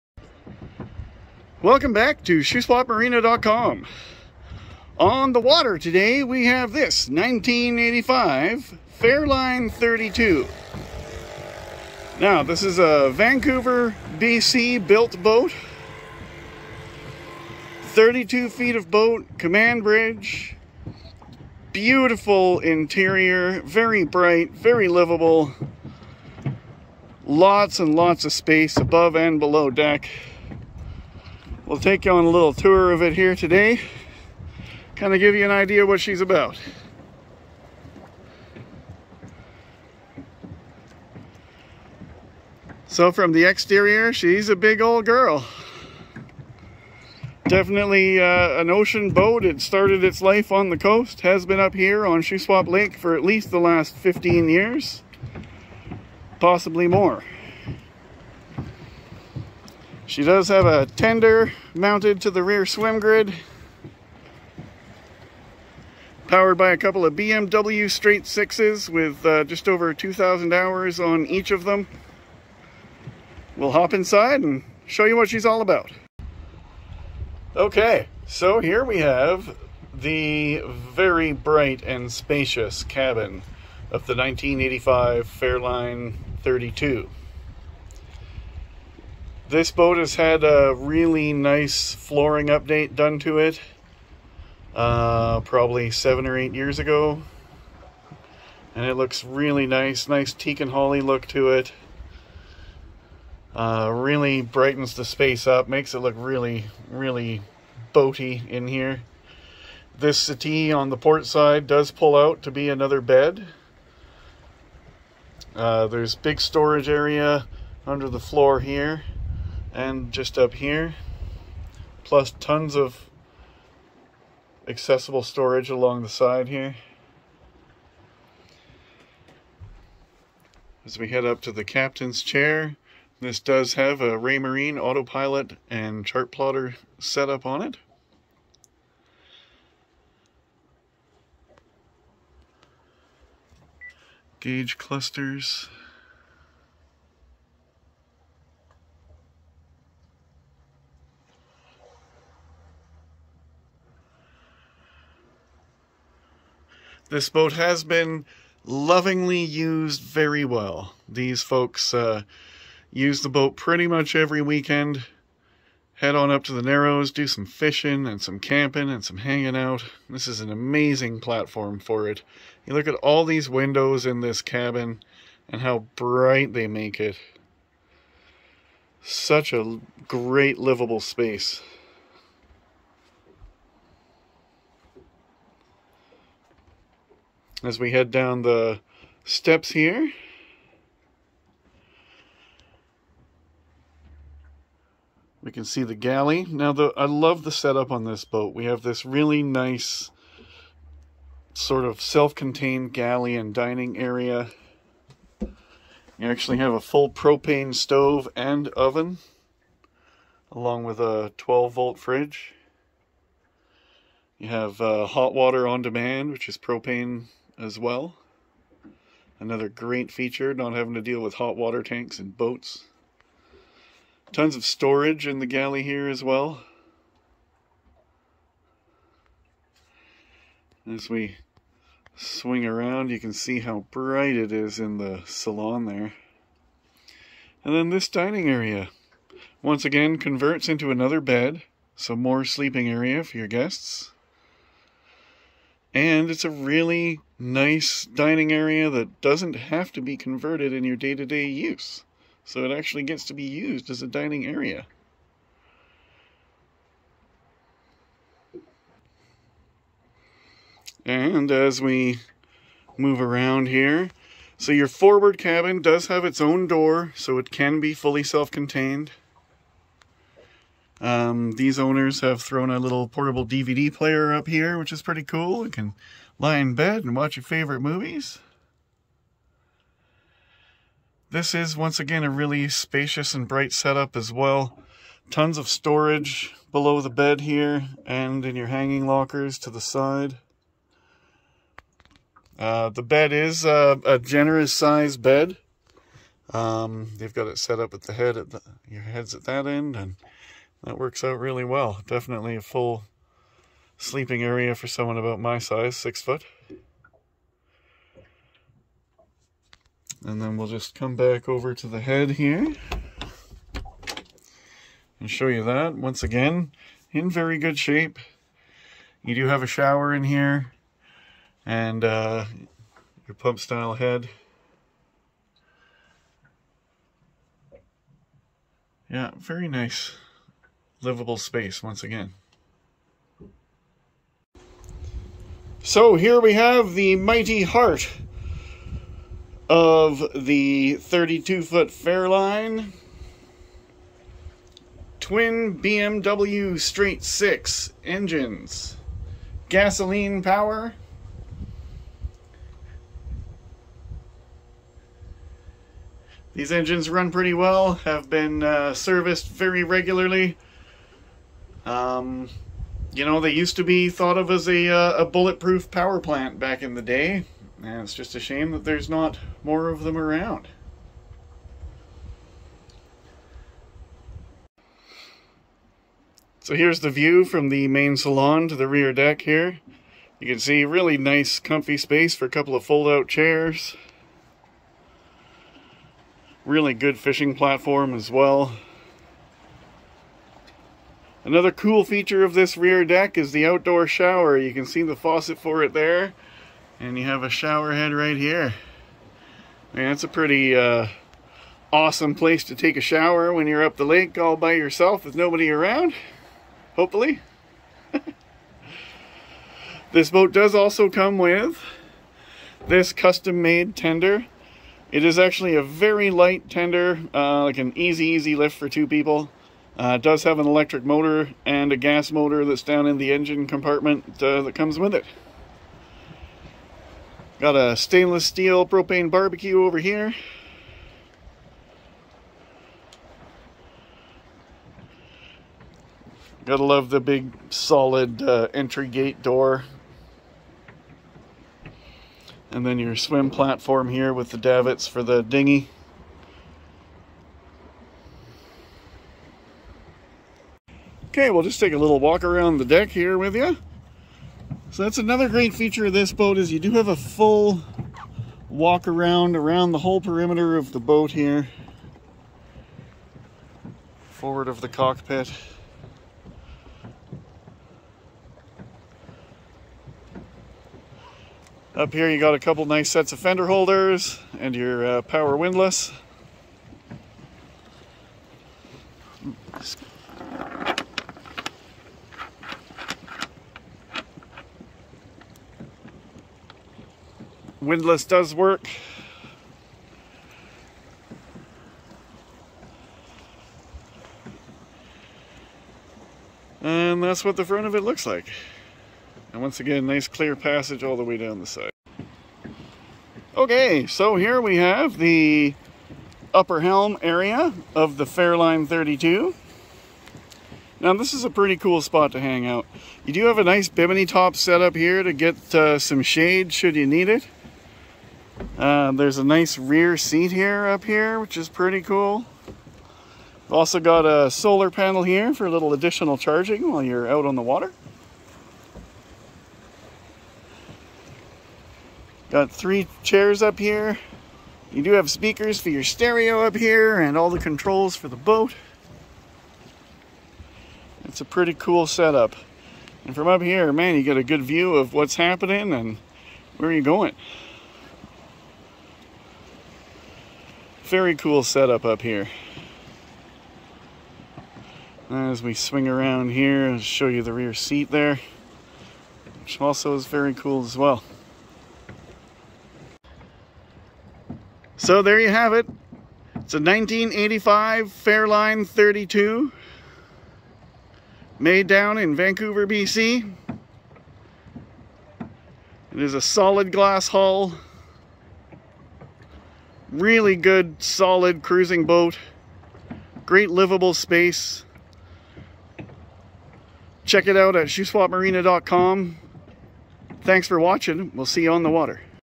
SOLD!! 1985 Fairline 32 Command Bridge - Twin BMW B220
SOLD!!
This is a very well built cruiser, made in Vancouver, BC! The bright and airy cabin is a major selling point for this boat as well as the general layout. The forward cabin has a door for great privacy, and aft of that is the full head with shower. There is a nice size galley and dining area that converts to more sleeping area if needed. There's a propane stove and oven in the galley along with hot water on demand that's also propane fired. There are 2 helm stations, with auto pilot and chart plotter GPS on the lower station. The engines run and drive just fine and have always been professionally serviced, they have about 1650 hours on them each. The main salon area is very bright with huge windows and a couch that converts into another double bed to make for a total of 6 sleeping capacity. The floors in the main cabin are an updated teak/holly laminate and make for a nice clean and nautical look. The upper station has a bimini top and seating for 3-5 people. This boat comes with a custom made dinghy/tender with both electric and small gas outboards. There is a diesel heater system on board for those cold nights and early mornings!

for more pics and a walkaround video.

This is a consignment sale. No trades, thank you. We will not include tax in any negotiations.